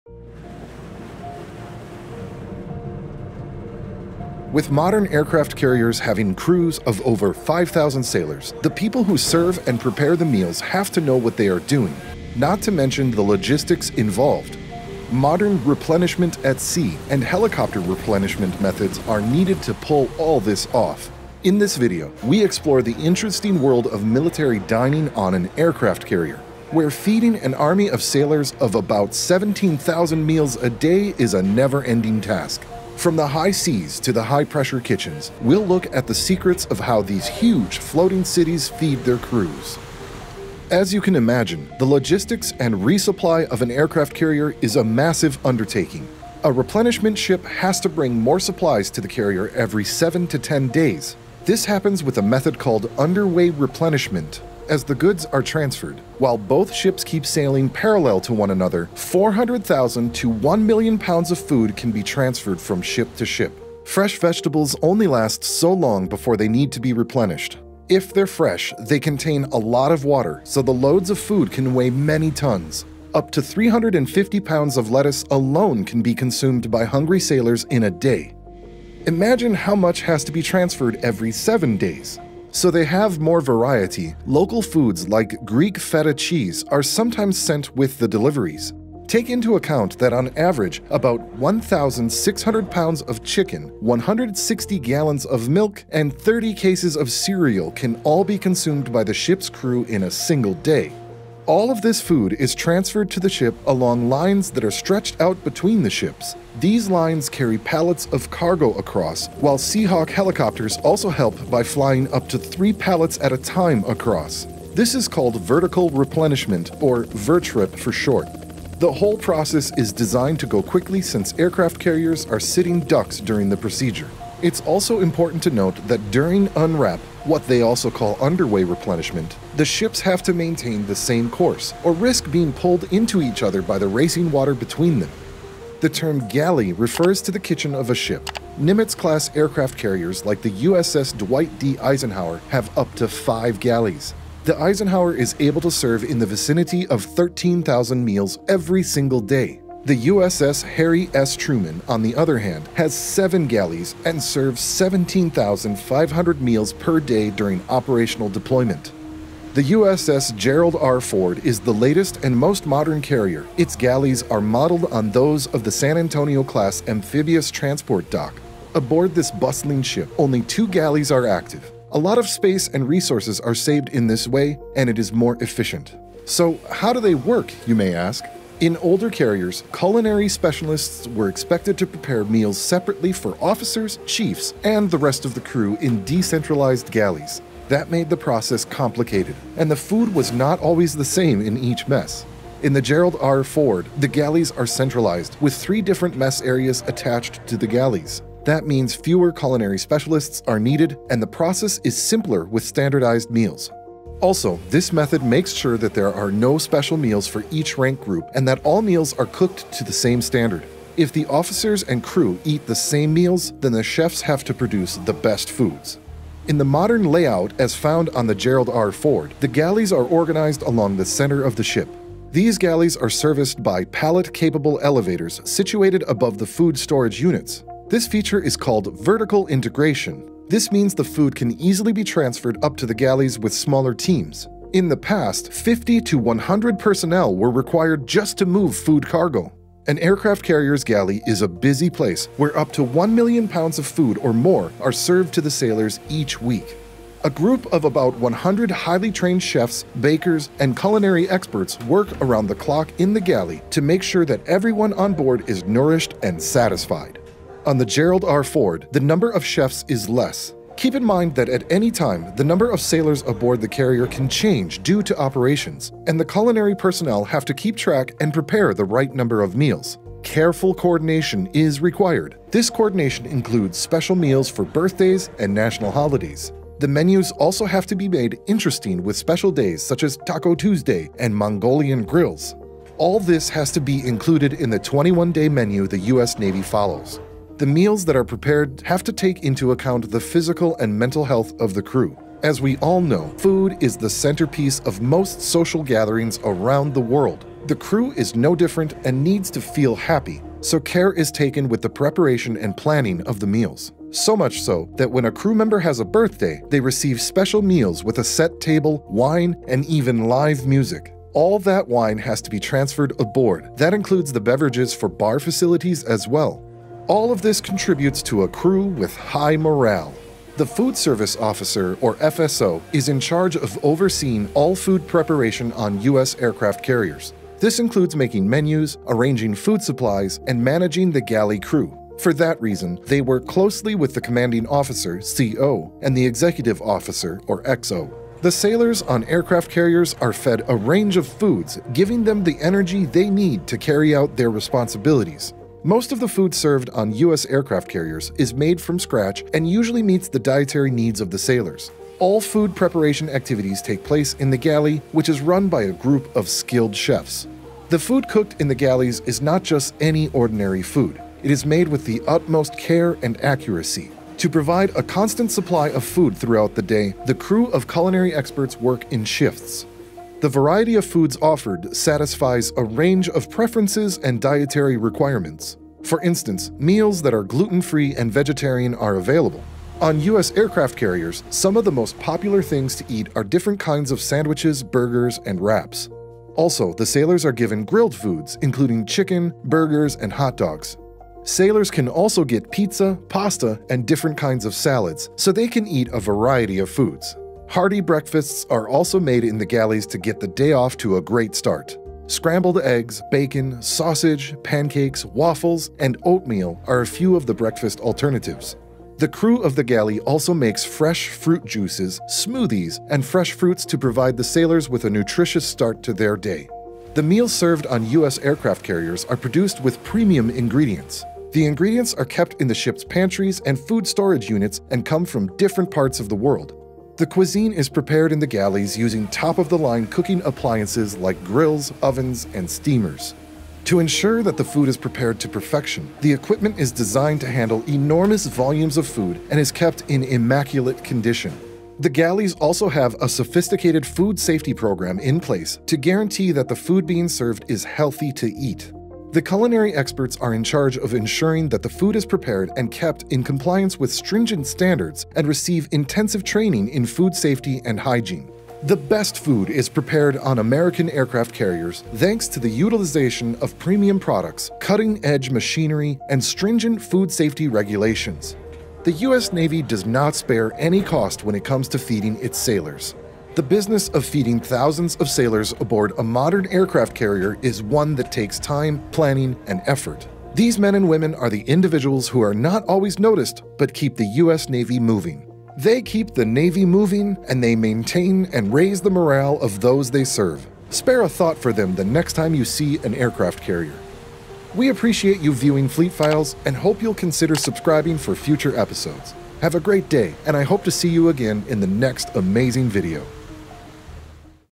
Feeding a Floating City: How 114 Cooks Make 17,000 Daily Meals For Navy Sailors
As you can imagine, the logistics and resupply of an aircraft carrier is a massive undertaking. A replenishment ship has to bring more supplies to the carrier every seven to ten days. This happens with a method called "underway replenishment." During one such replenishment, while both ships keep sailing parallel to one another, 400,000 to 1 million pounds of food can be transferred.

In the video, we explore the interesting world of military dining on an aircraft carrier, where feeding an army of sailors about 17,000 meals a day is a never-ending task. From the high seas to the high-pressure kitchens, we'll look at the secrets of how these huge floating cities feed their crews.